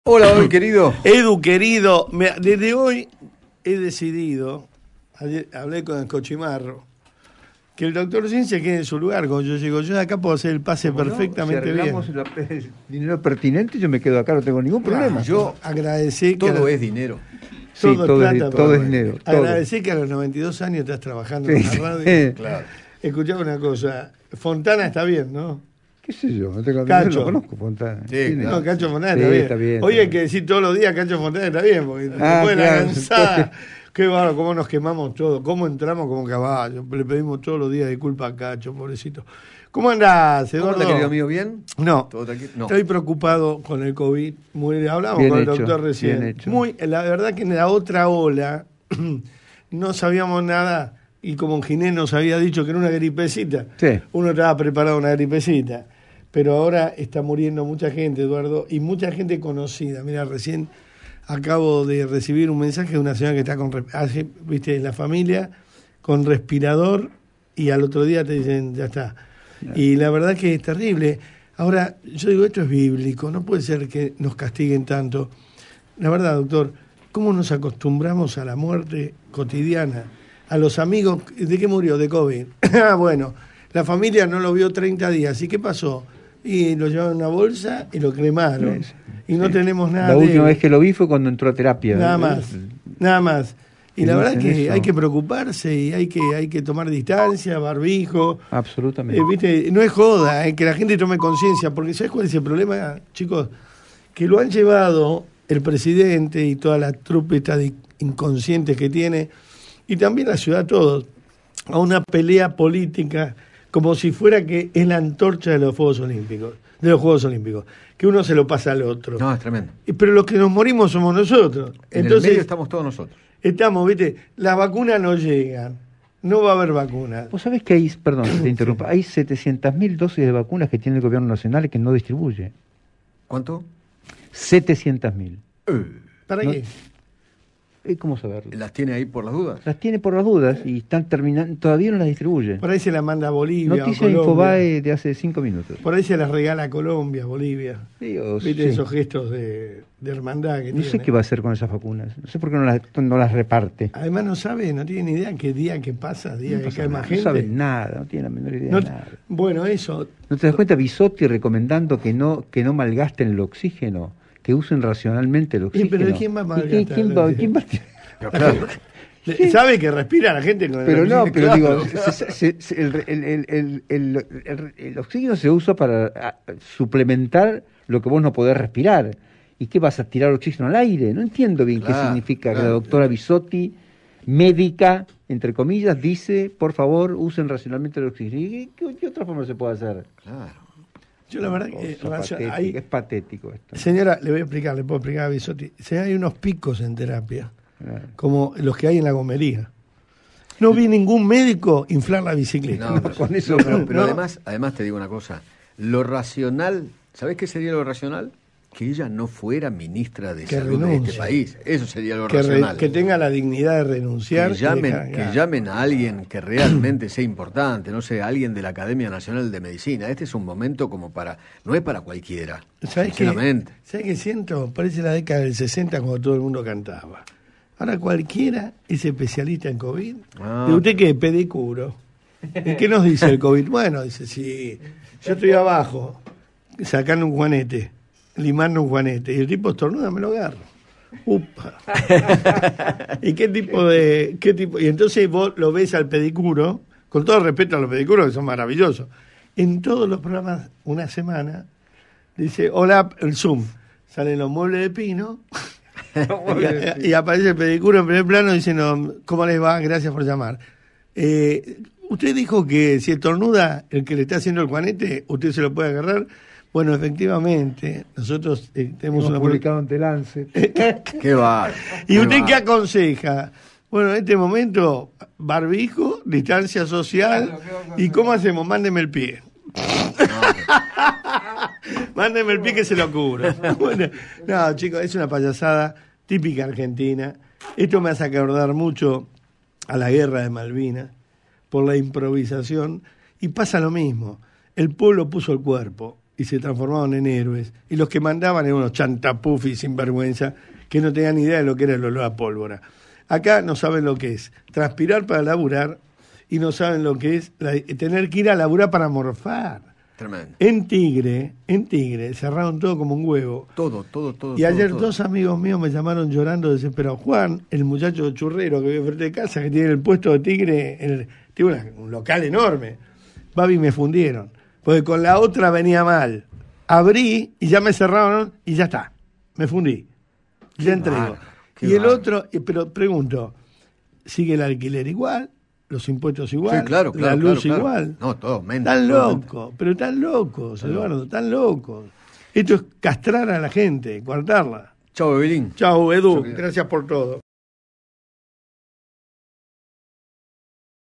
El pase entre Eduardo Feinmann y Baby Etchecopar
Escuchá el pase del mediodía entre Eduardo Feinmann y Baby Etchecopar en radio Rivadavia.